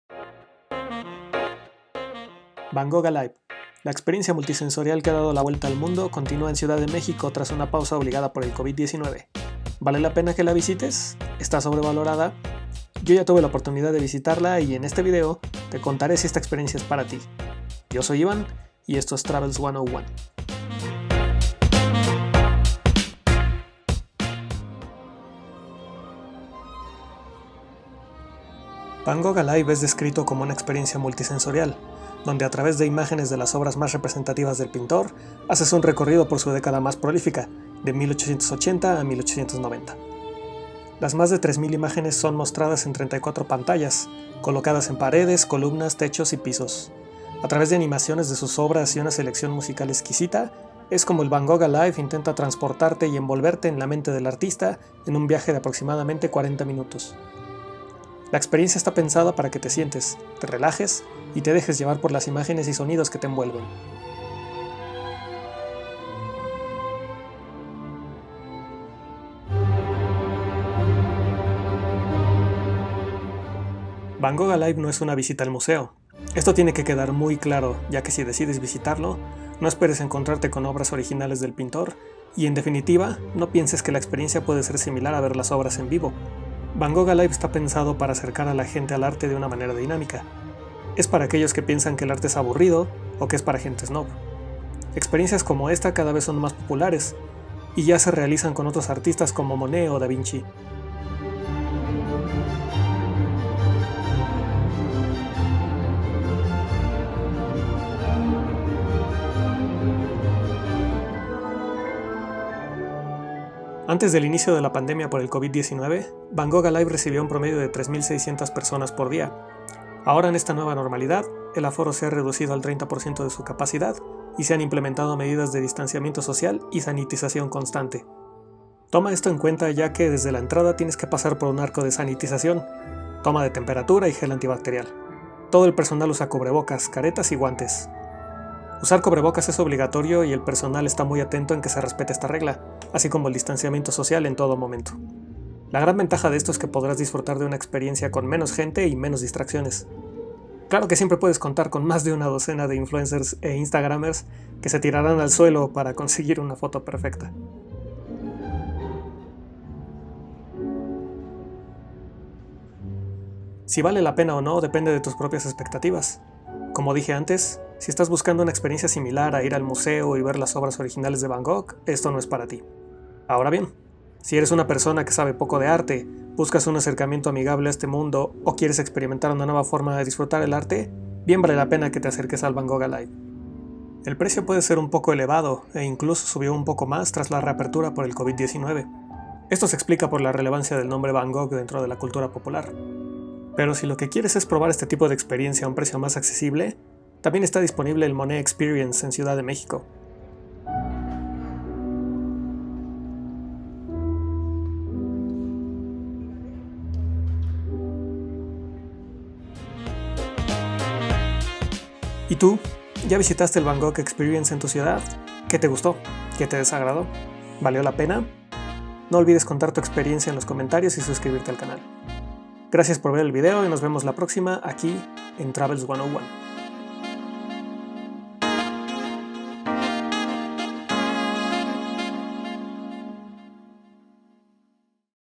Van Gogh Alive ¿Experiencia sobrevalorada?
Visité el Van Gogh Alive: The Experience para ver por qué tanto escándalo y te digo si esta experiencia es para ti. También te cuento sobre las medidas de seguridad implementadas por el COVID-19.